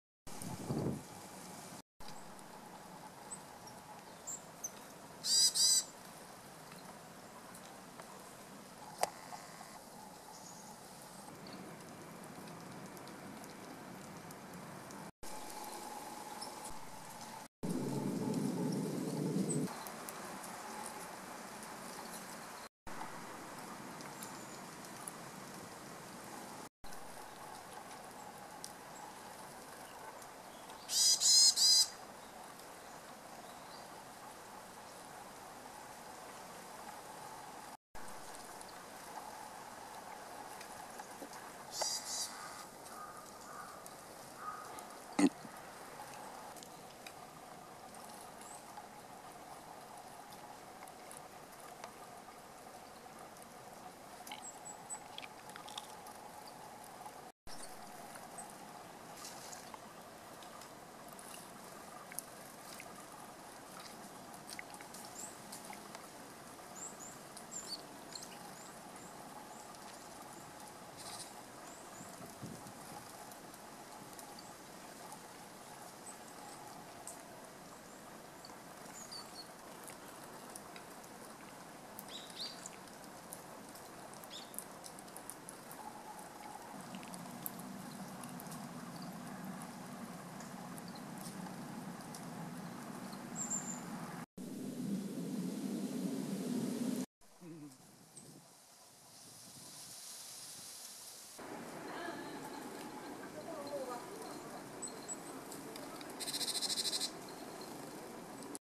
野鳥：Ｓ緑地の餌台に／シジュウカラ・ヤマガラ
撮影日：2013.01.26
撮影機器：Panasonic LUMIX FZ200
博物館の階段を上ってＳ緑地の入り口にある餌台に
シジュウカラやヤマガラたちが群でやって来ました。
カワラヒワも近くまで来ましたが餌台には来ません。